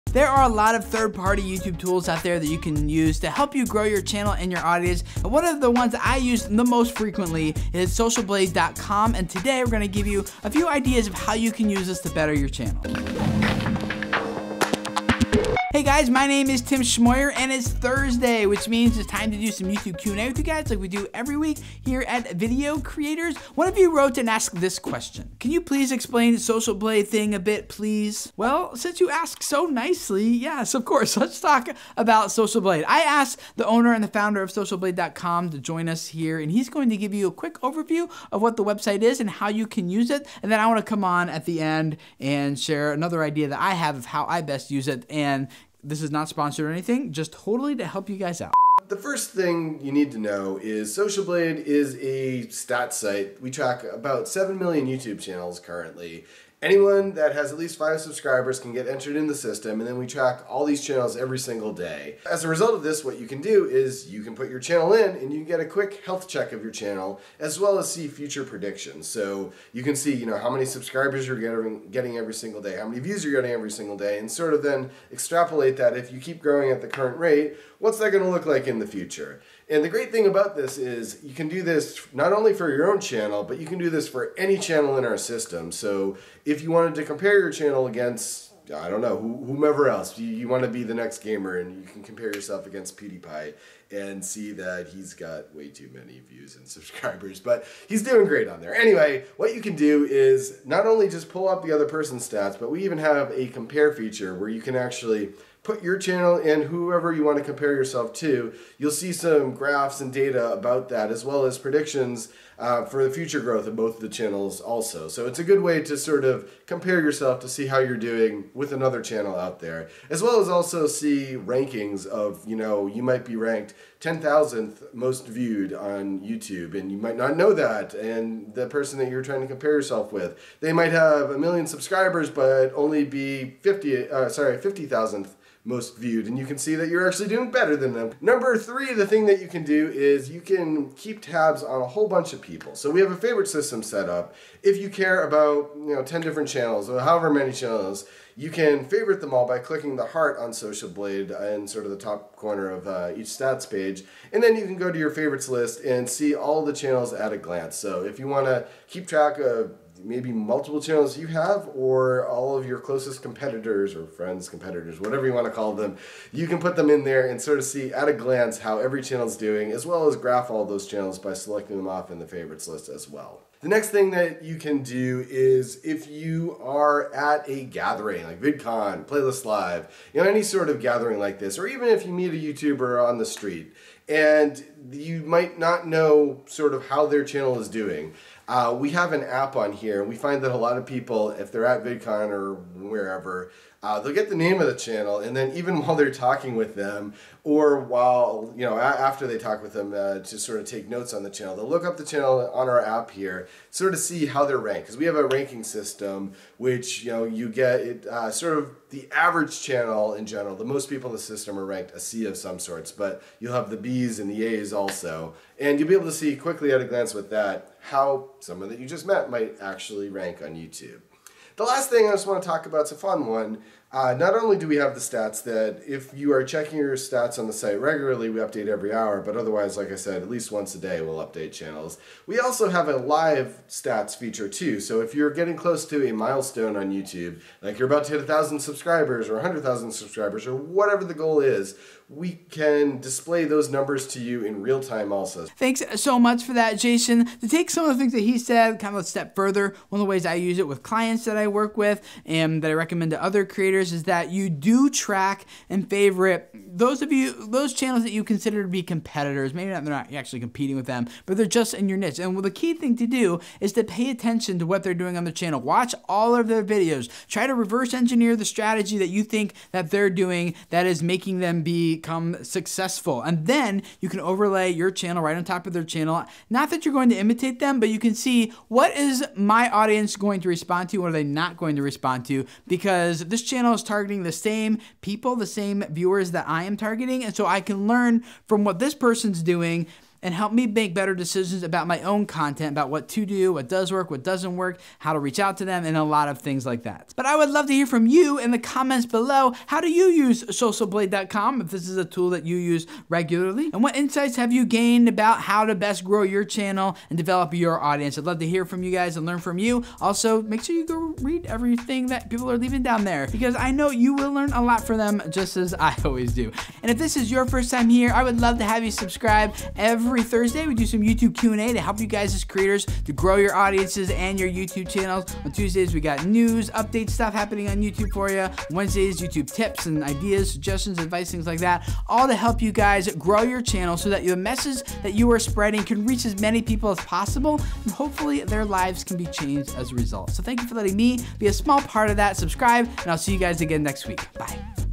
5 FREE Ways to Improve your Channel with Socialblade
Socialblade is a great free tool you can use to grow and improve your YouTube channel. By viewing both your YouTube statistics and the stats of other YouTube channels you can see what works and what doesn't work in order to make better decisions about your videos. The founder and owner of Socialblade, Jason Urgo, joins us to share 5 ideas of how to use Socialblade as a tool for improving your YouTube strategy.